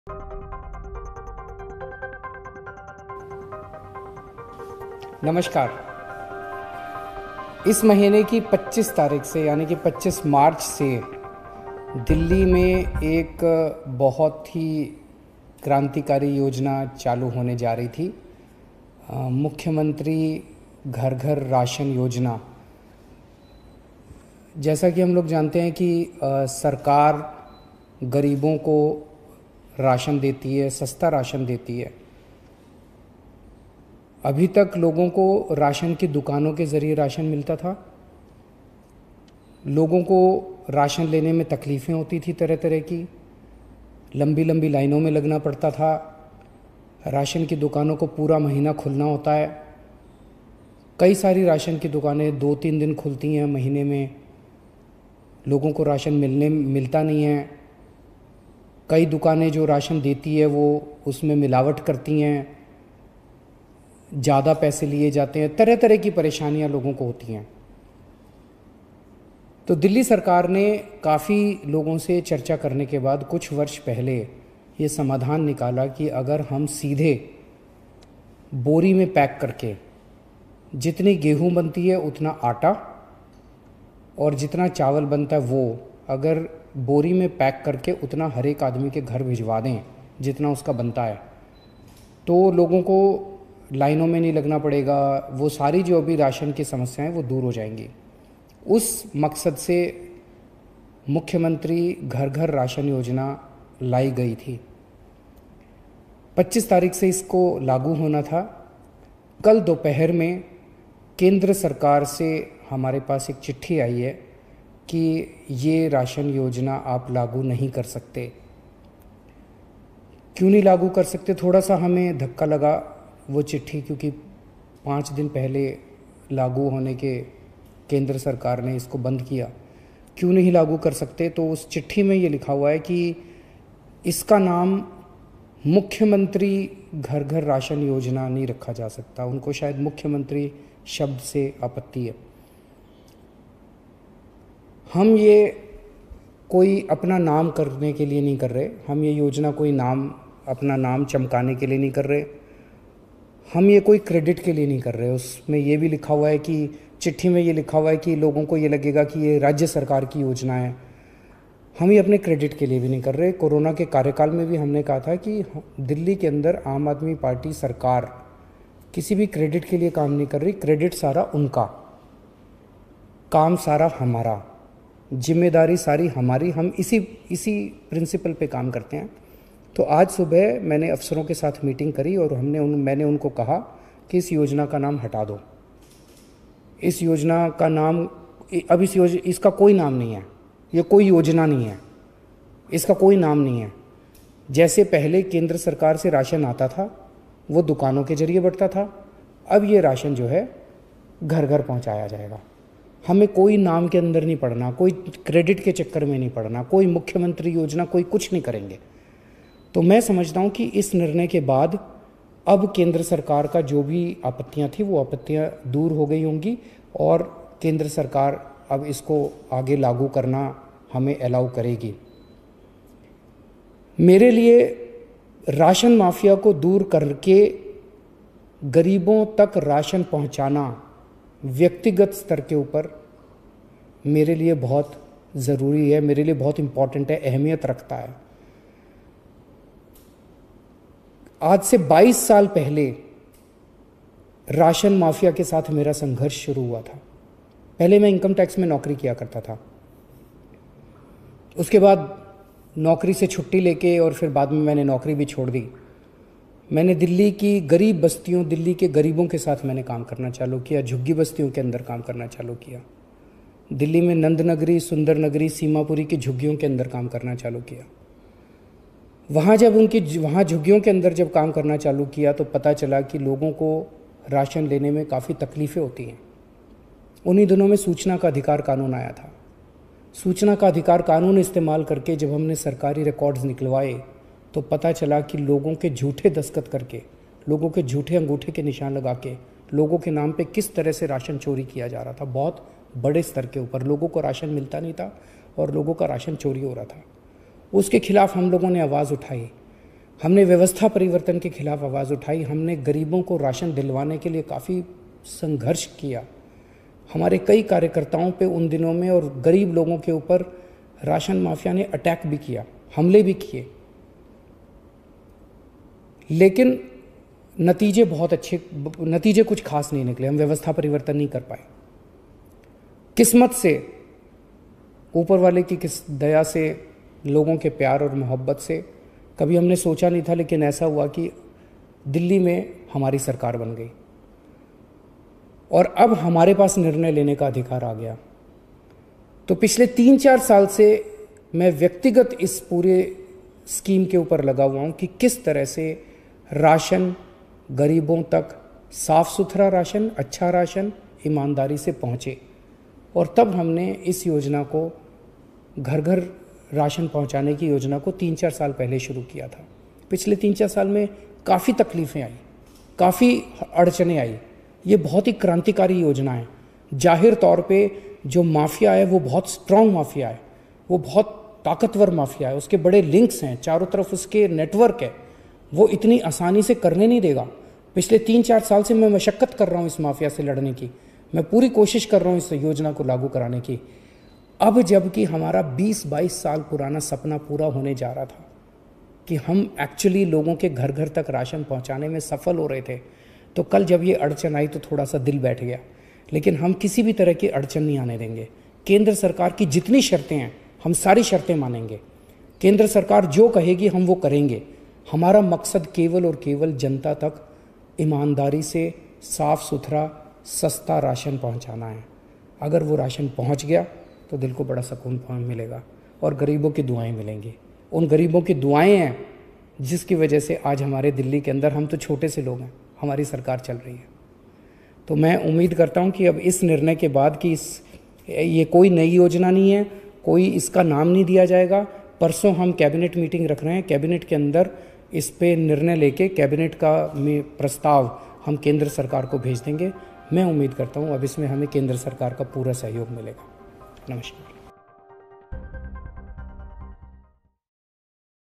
Doorstep ration delivery scheme in Delhi will have no name, says Kejriwal
Delhi Chief Minister ArvindKejriwal on Saturday said the DoorstepRation delivery scheme of his government would not have any name, a day after the Centre red flagged 'Mukhya Mantri Ghar Ghar Ration Yojna' which was scheduled to start from March 25.